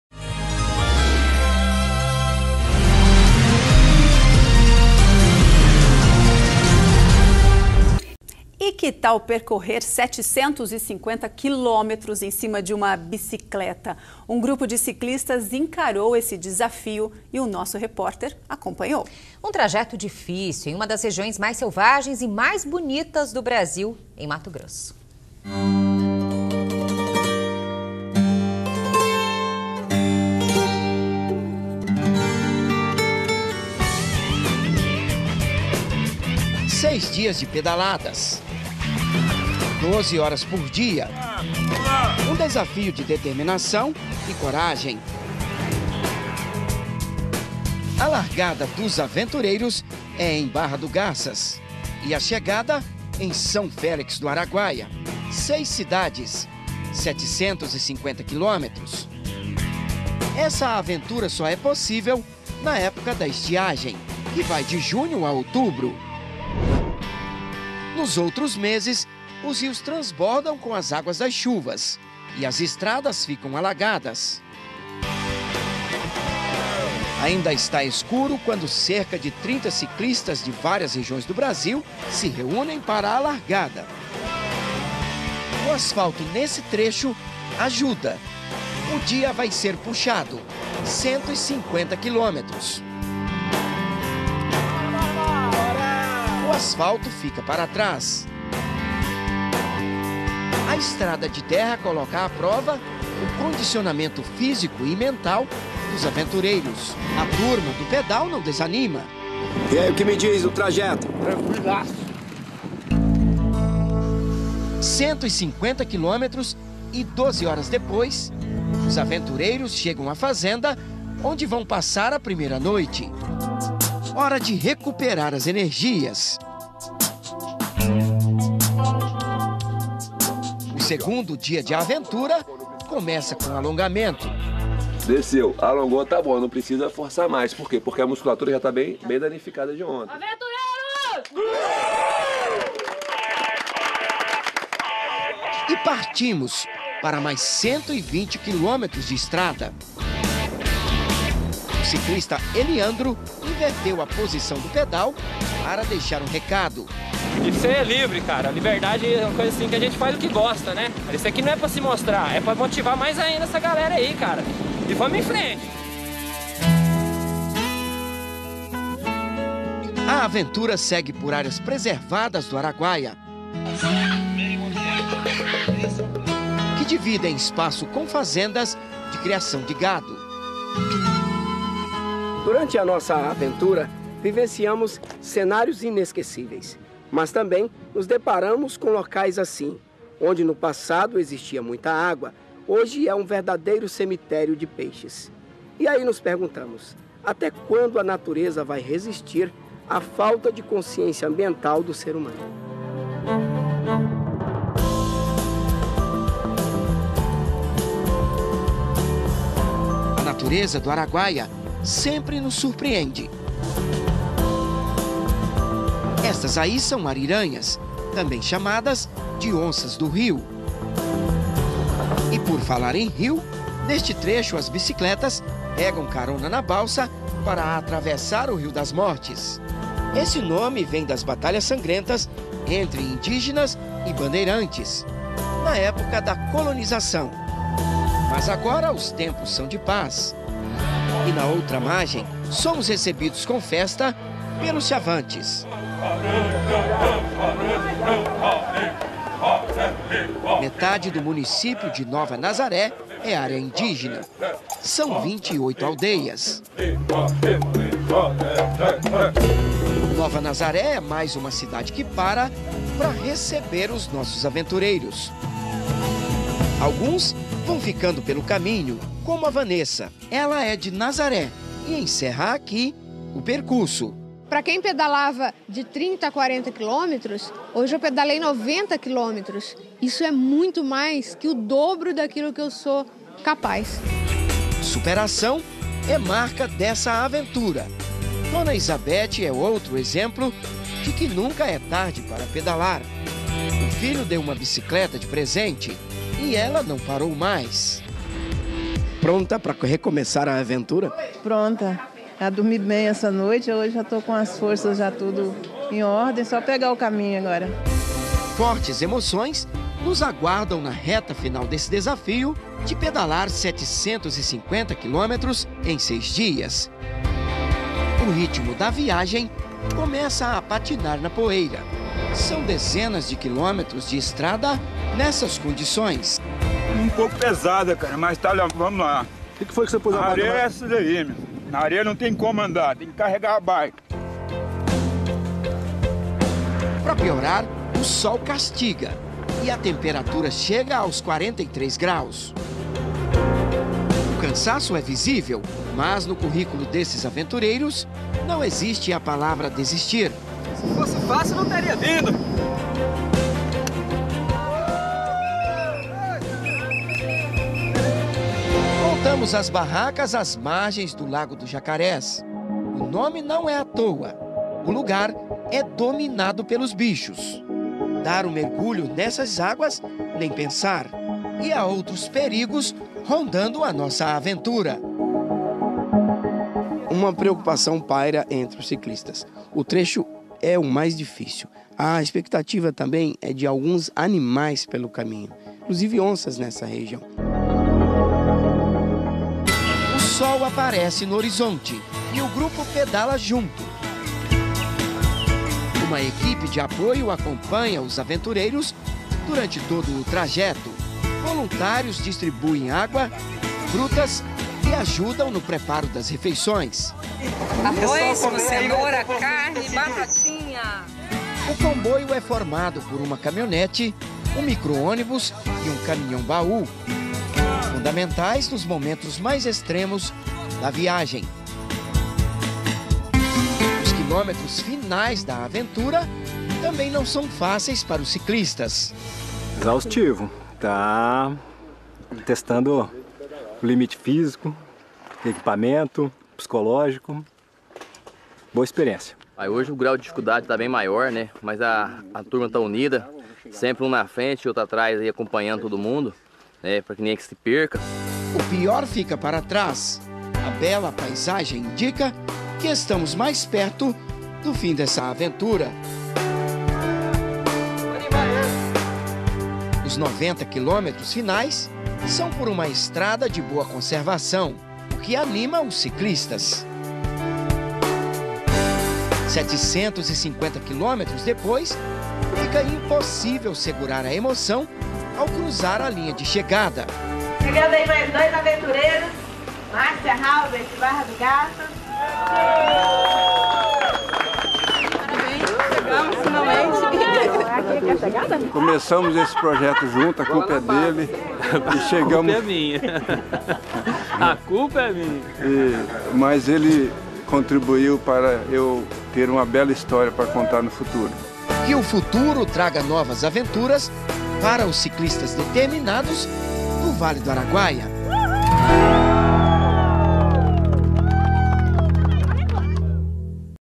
750 km de Bike por uma das regiões mais selvagens do Brasil!!
O desafio foi denominado de Aventura Tour que reuniu ciclista de varia regiões do Brasil, o percurso é entre Barra do Garças e São Felix do Araguaia - MT.